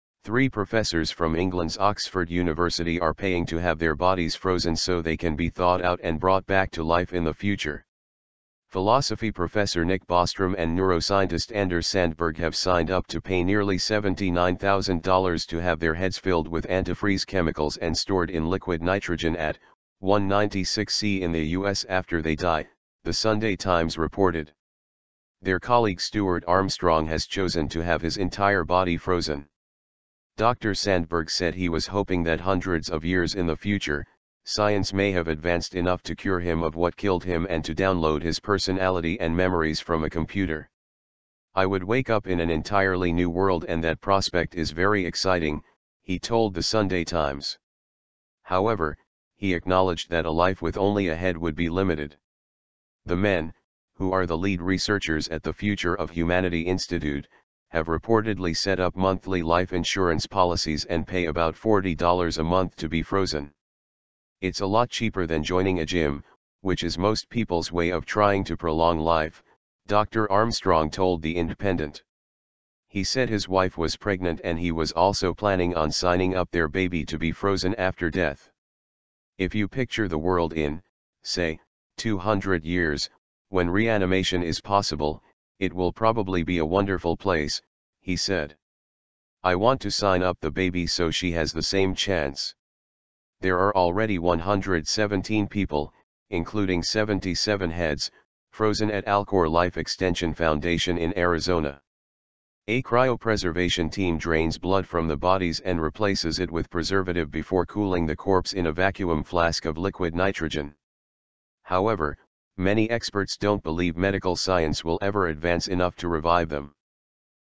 3 Oxford University Professors Pay For Being Frozen And Brought Back To Life
Three professors from England's Oxford University are paying to have their bodies frozen so they can be thawed out and brought back to life in the future.

 Philosophy professor Nick Bostrom and neuroscientist Anders Sandberg have signed up to pay nearly $79,000 to have their heads filled with "antifreeze" chemicals and stored in liquid nitrogen at -196C in the US after they die, The Sunday Times reported.

Their colleague Stuart Armstrong has chosen to have his entire body frozen.

Dr Sandberg said he was hoping that hundreds of years in the future, science may have advanced enough to cure him of what killed him and to download his personality and memories from a computer.

"I would wake up in an entirely new world and that prospect is very exciting," he told The Sunday Times.

However, he acknowledged that a life with only a head would be "limited".

The men, who are the lead researchers at the Future of Humanity Institute, have reportedly set up monthly life insurance policies and pay about $40 a month to be frozen.

"It's a lot cheaper than joining a gym, which is most people's way of trying to prolong life," Dr Armstrong told The Indpendent.

He said his wife was pregnant and he was also planning on signing up their baby to be frozen after death.

"If you picture the world in, say, 200 years, when reanimation is possible, it will probably be a wonderful place," he said.

"I want to sign up the baby so she has the same chance."

There are already 117 people, including 77 heads, frozen at Alcor Life Extension Foundation in Arizona.

A cryopreservation team drains blood from the bodies and replaces it with preservative before cooling the corpse in a vacuum flask of liquid nitrogen.

However, many experts don't believe medical science will ever advance enough to revive them.